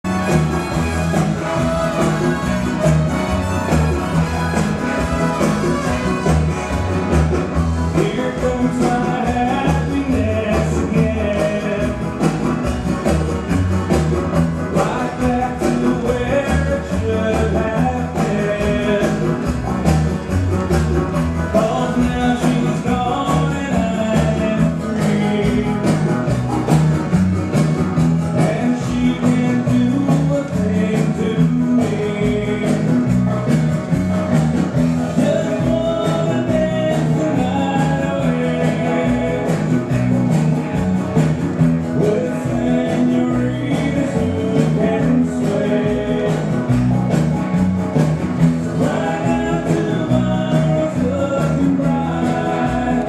SCOOTICH
UNE TRES BELLE DANSE EN COUPLE
QUI SE DANSE PARTOUT.